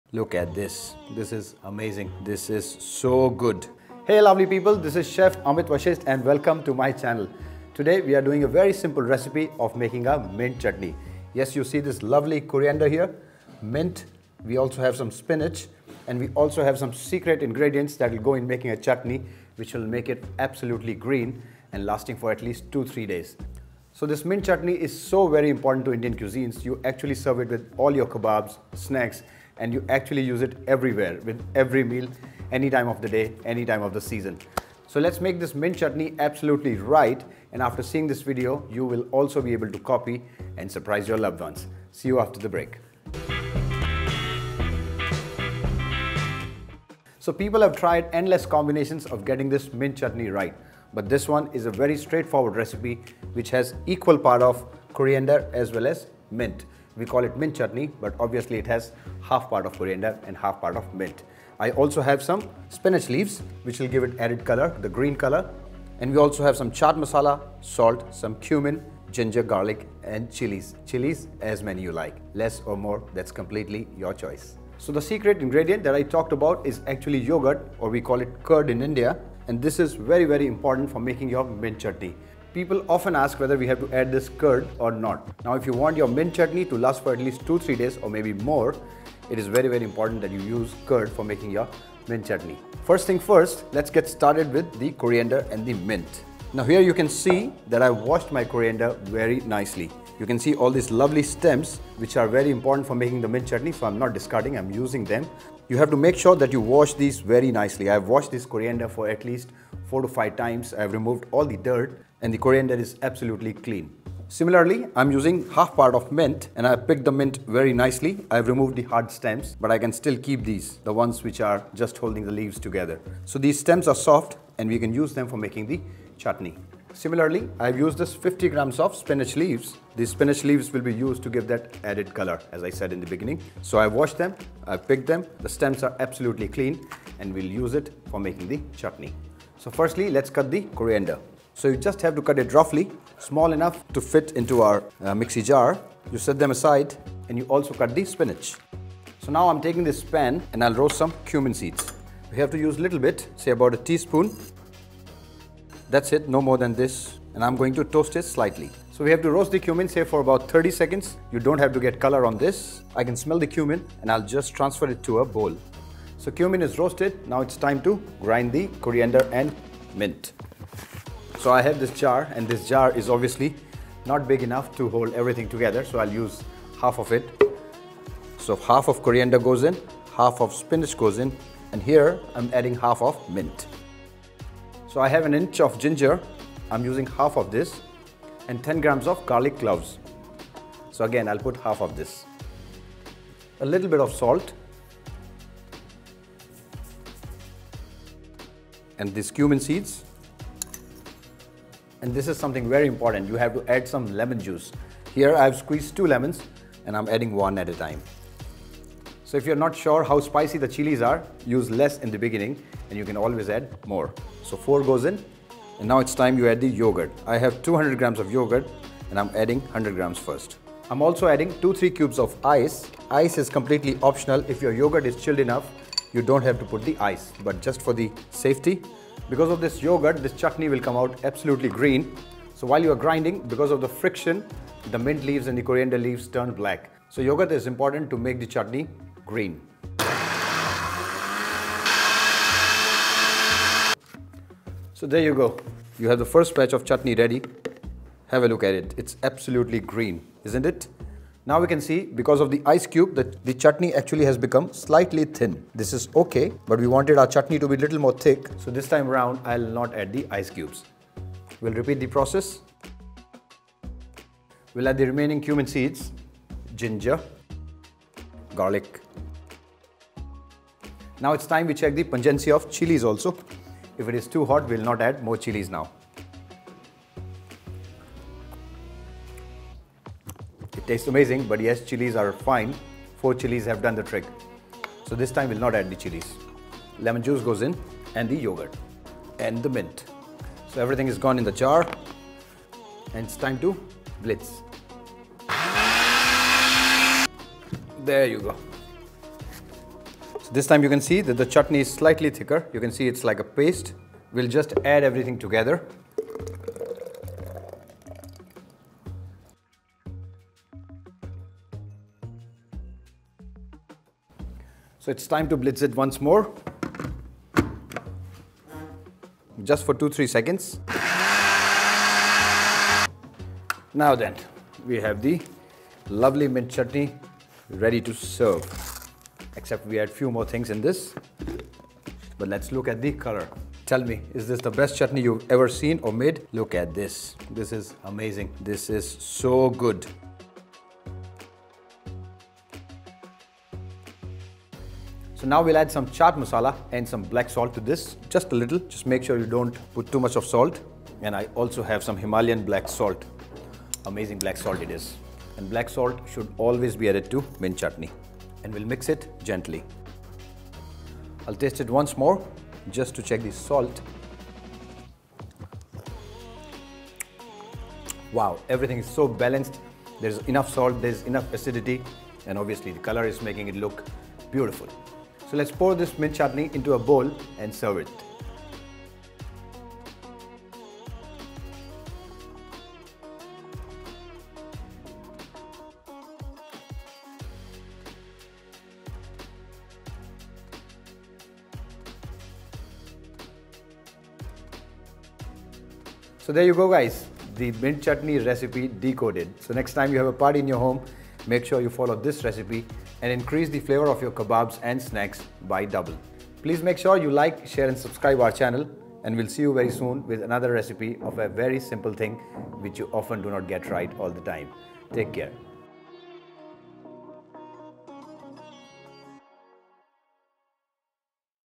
Mint Chutney | Perfect Recipe You Have Always Been Doing Wrong | Indian Chutney | Chef Amit Vashist
INGREDIENTS

Yogurt - 200 gms
Coriander - 100 gms
Mint - 100 gms
Spinach - 50 gms
Garlic - 10 gms
Cumin Seeds - 5 gms
Salt
Chaat Masala
Black Salt

KEEP COOKING AND KEEP LEARNING.

Credits
Forever Frame

Ravi Goswami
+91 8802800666